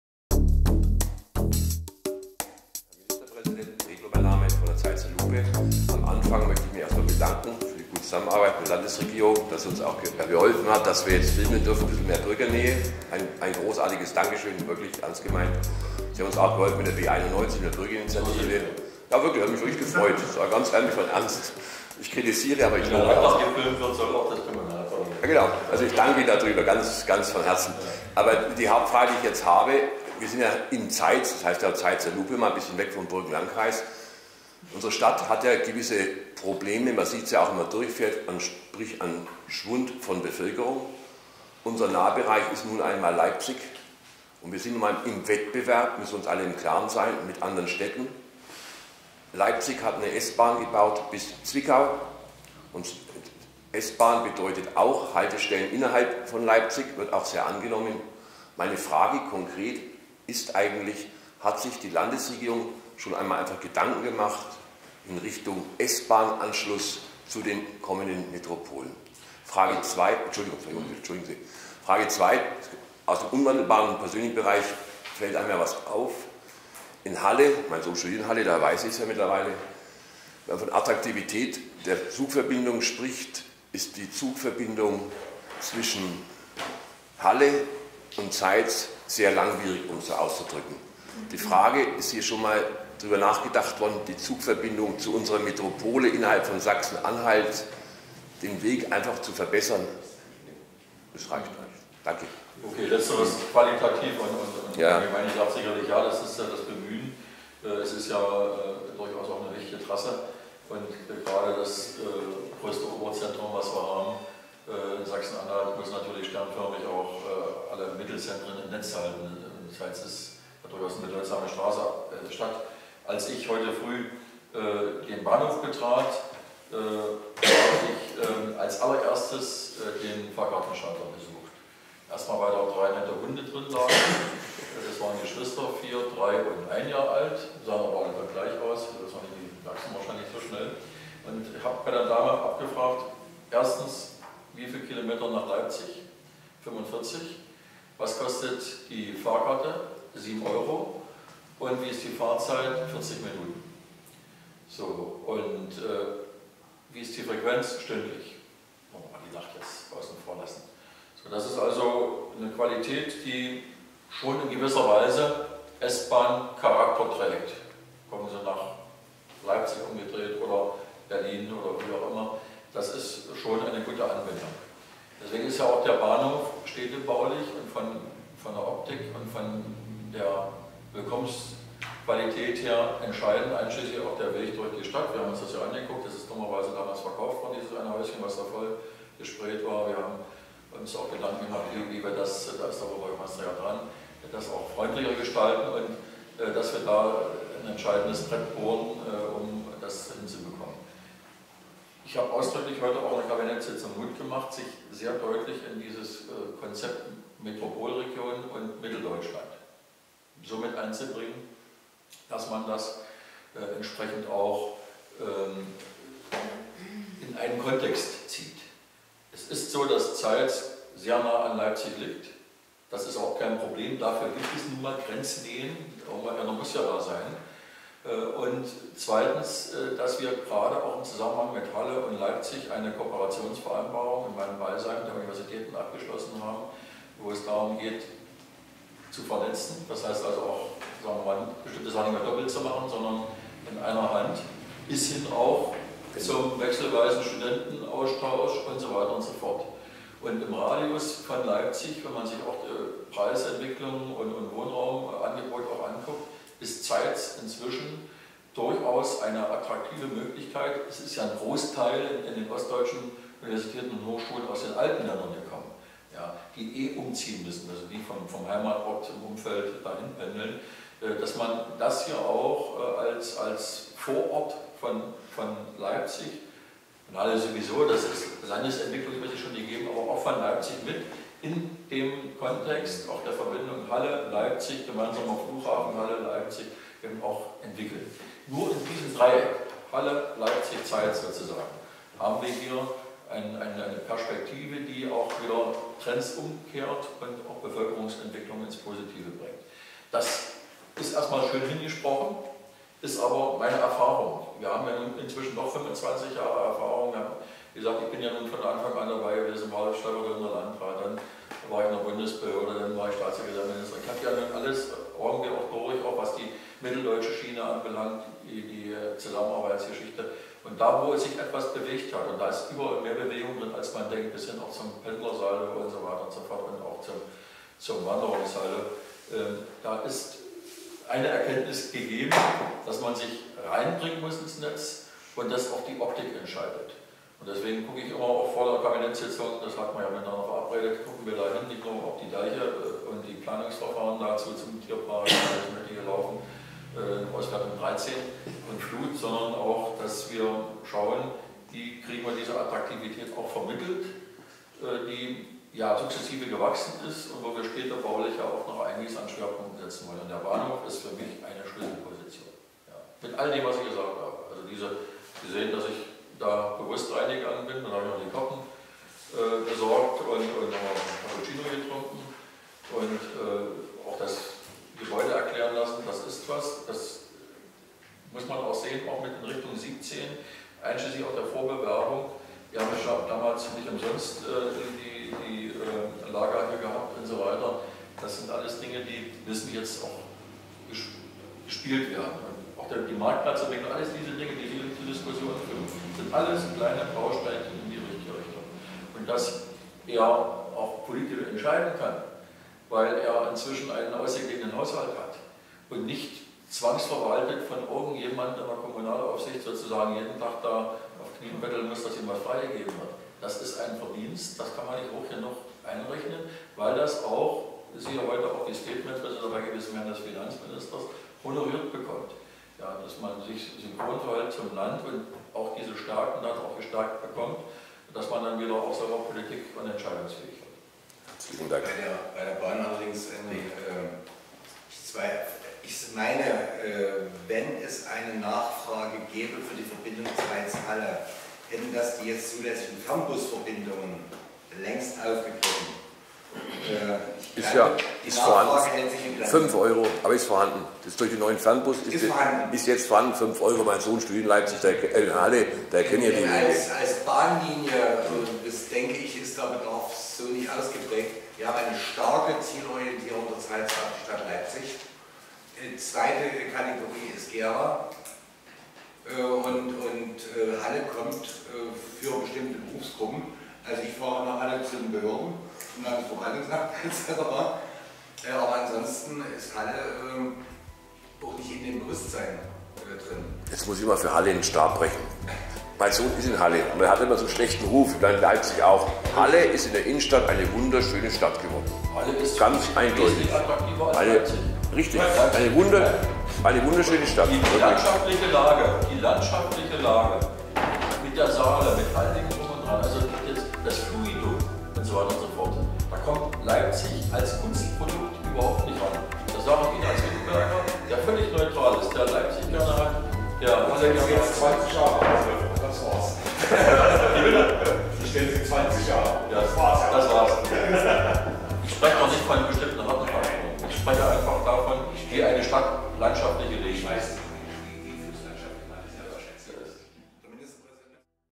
MP Dr.Reiner Hasseloff äußert sich zu S Bahn Pressefreiheit u der Schönheit von Halle
Die Landesregierung von Sachsen-Anhalt kam am Dinstag den 21. Oktober 2014 zu einer auswärtigen Kabinettssitzung in Zeitz zusammen. Im Nachgang der Sitzung wurde zu einer Pressekonferenz geladen. Wir nahmen als Zeitzer Lupe daran teil und beteiligten uns mit Fragen zu Zeitz bzgl. S-Bahn Leipzig, Verbindung nach Halle und weiteres. Unser Ministerpräsident antwortete sehr ausführlich und vermittelte ein starkes Interesse für unser Zeitz, was uns sehr erfreute.Die gesamte Pressekonferenz laden wir auch noch hoch. MfG W.Dreblow